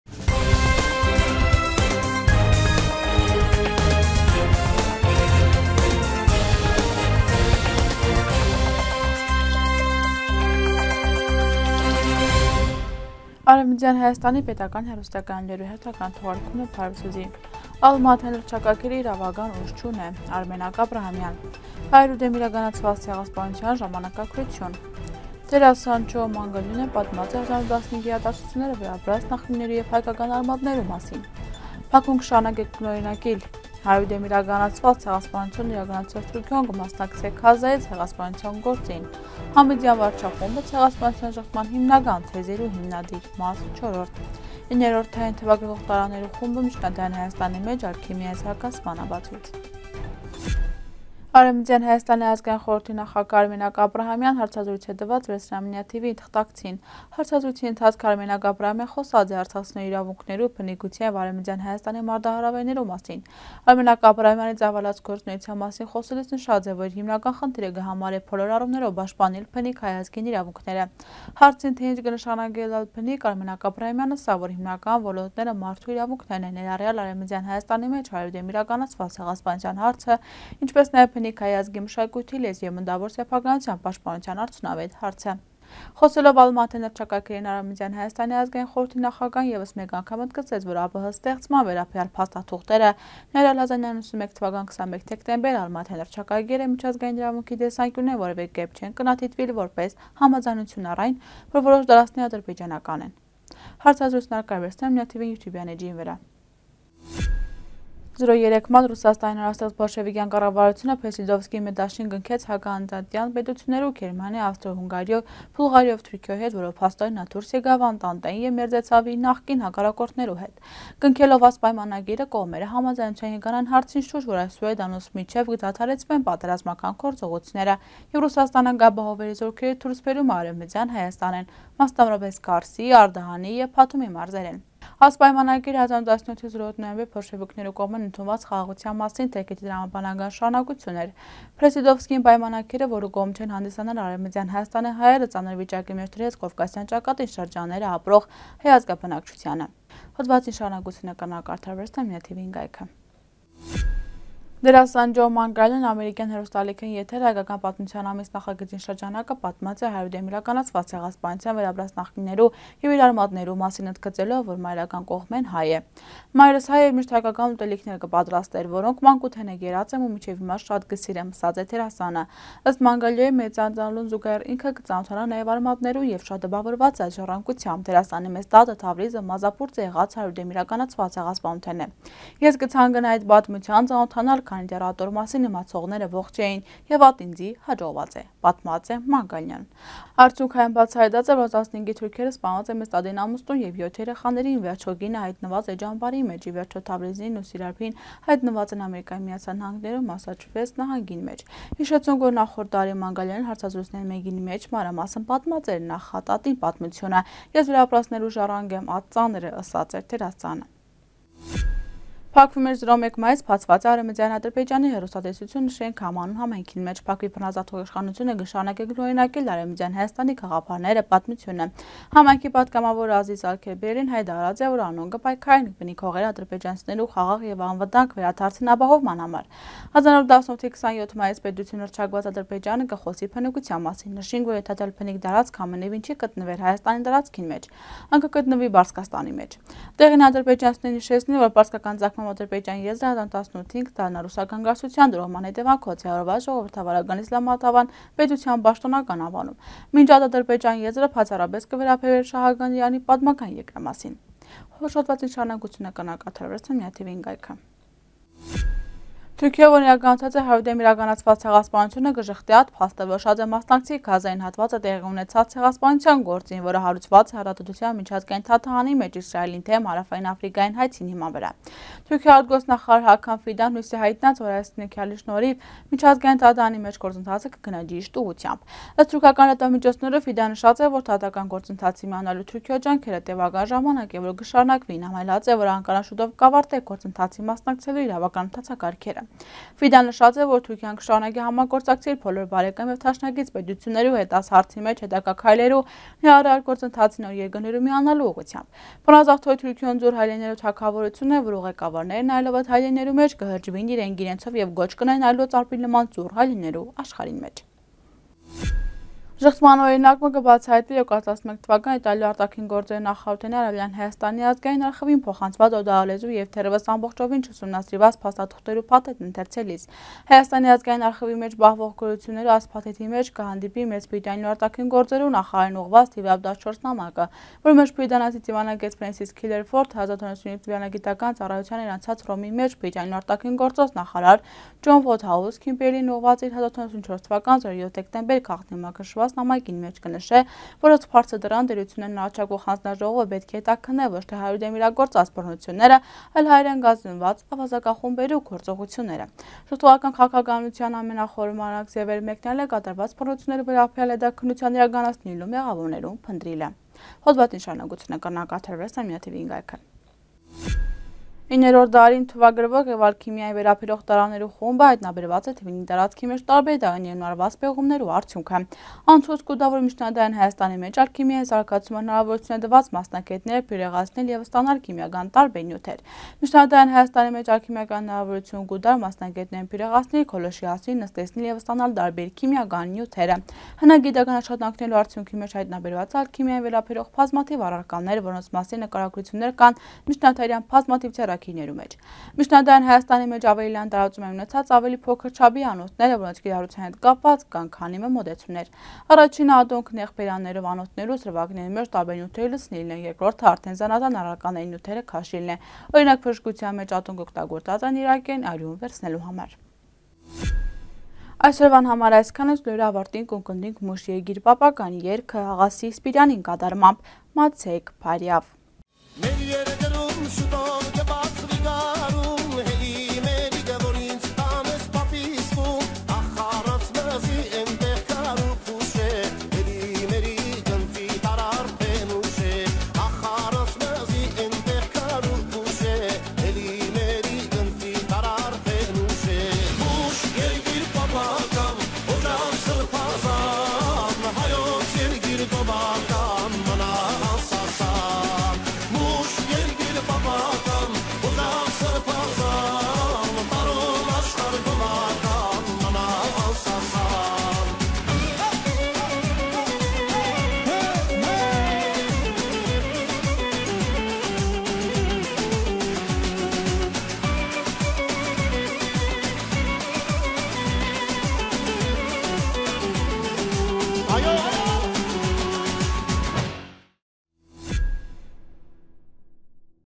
Ալմա-Աթային հռչակագիրը իրաւական ուժ չունէ․ Արմենակ Աբրահամեան
1. Ալմա-Աթային հռչակագիրը իրաւական ուժ չունէ․ Արմենակ Աբրահամեան
2. Հայերու դէմ իրականացուած ցեղասպանութեան ժամանակագրութիւն
3. Դերասան Ջո Մանգանյելոն պատմած է 1915-ի իրադարձութիւնները վերապրած նախնիներու եւ հայկական արմատներու մասին
4. Բաքուն կը շարունակէ կրկնօրինակիլ
5. Հայերու դէմ իրականացուած ցեղասպանութիւնն իրականացուցած Թիւրքիոն կմասնակցէ Գազայի ցեղասպանութեան գործին
6. Յամիդեան վարչախումբը՝ ցեղասպանութեան ժխտման հիմնական թեզերու հիմնադիր (Մաս չորրորդ)
7. 9-րդ դարին  թուագրուող տարաներու խումբը միջնադարեան Հայաստանին մէջ ալքիմիային զարգացման ապացոյց